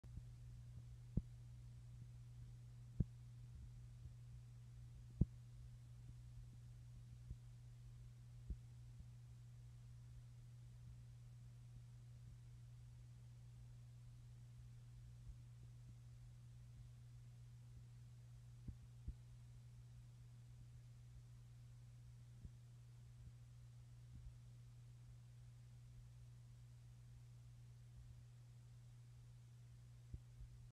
Unknown favia coral
Im not sure what this is, may not even be a favia.